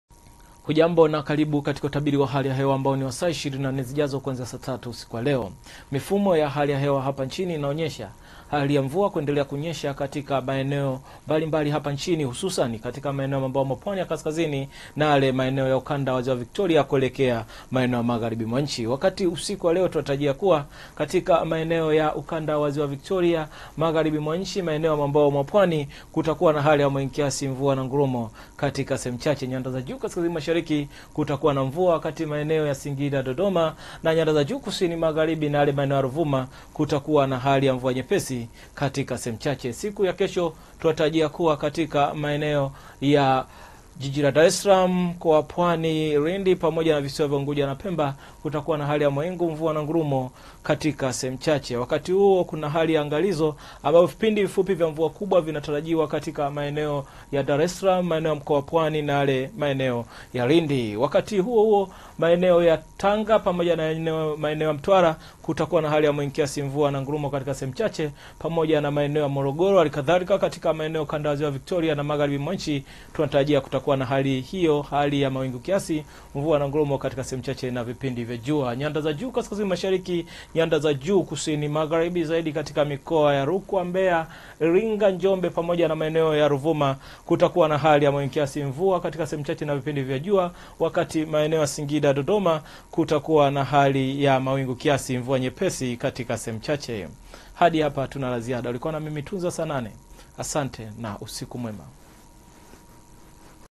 UTABIRI WA HALI YA HEWA LEO USIKU 03-04-2021
Utabiri wa hali ya hewa wa saa 24 zijazo kuanzia saa 3:00 usiku wa leo tarehe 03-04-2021 unawasilishwa na mchambuzi Tunza Sanane.